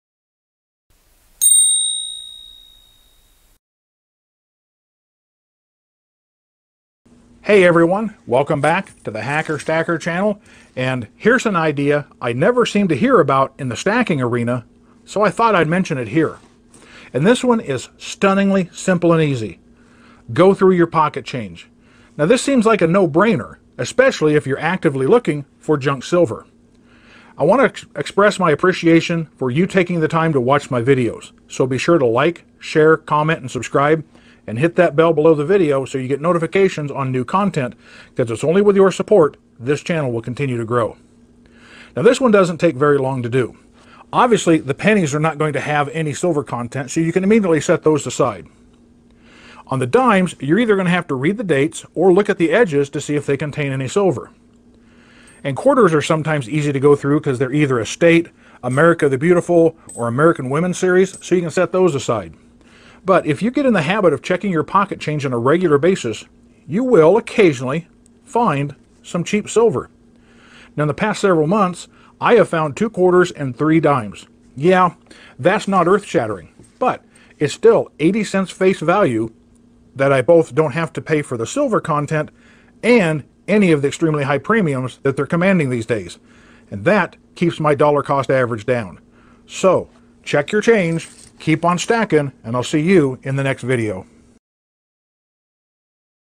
Get Junk Silver for Face Value!
How to get 90 and 40% silver for your stack without paying spot plus premium.